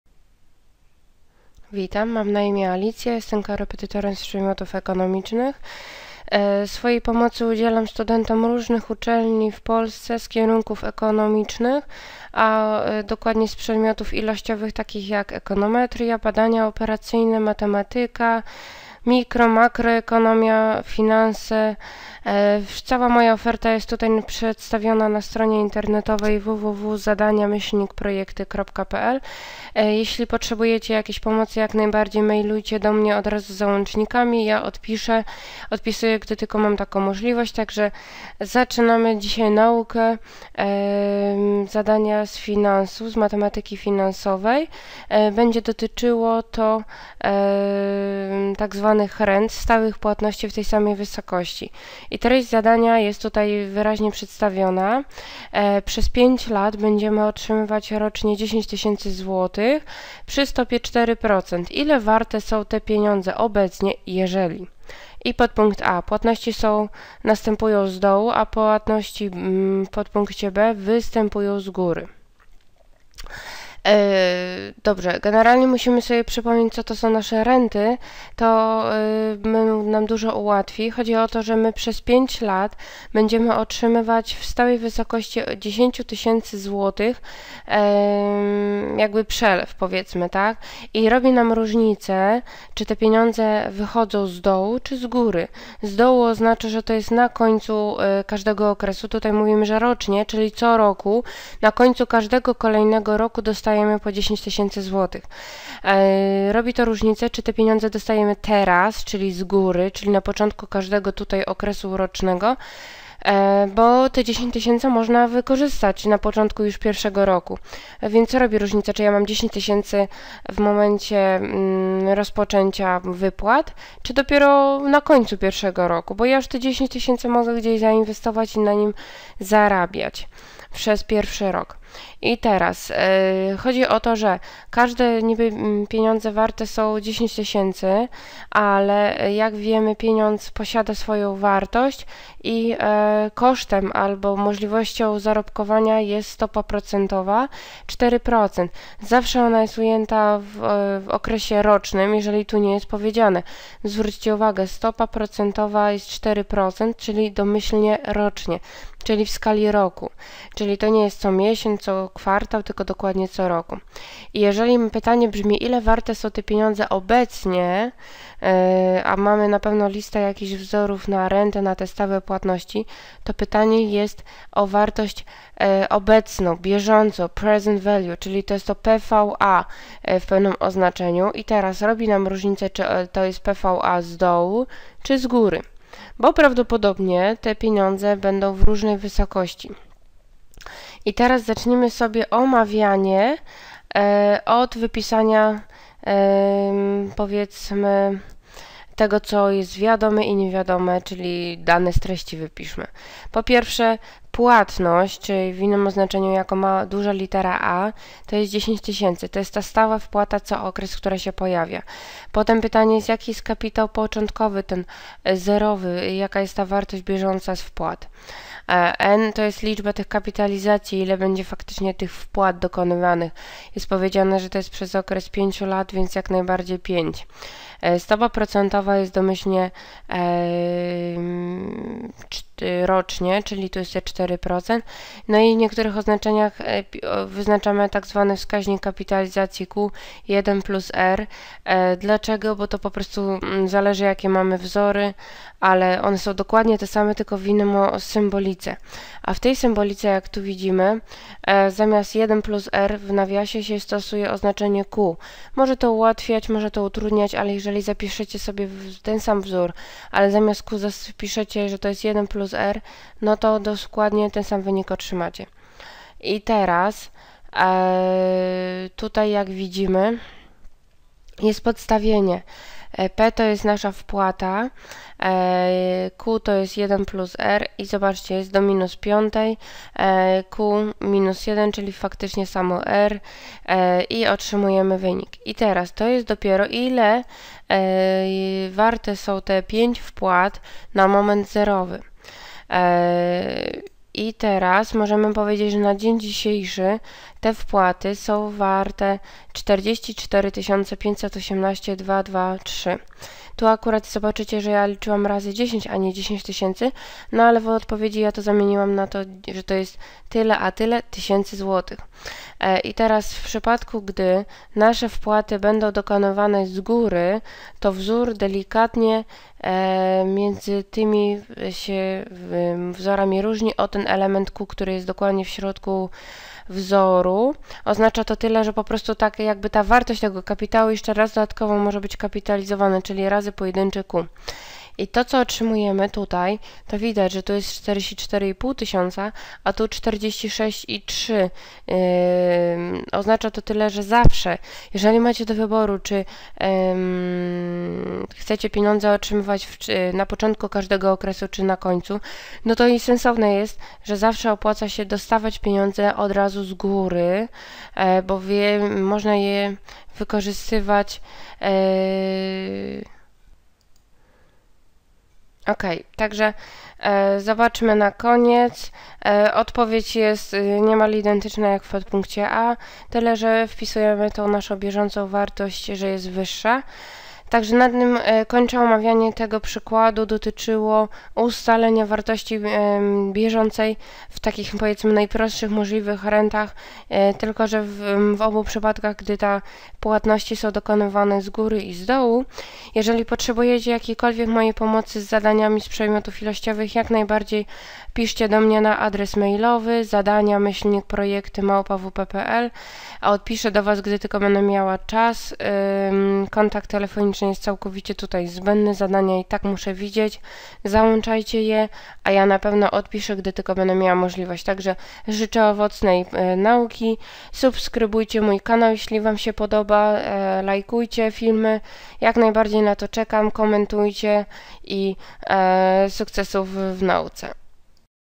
Renty - ustalanie wartości bieżącej PVA - podstawy - płatność z góry i z dołu
Przez 5 lat będziemy otrzymywać rocznie 10 tys. PLN przy stopie 4%. Ile warte są te pieniądze obecnie, jeżeli:
(a) płatności następują z dołu,
(b) płatności występują z góry?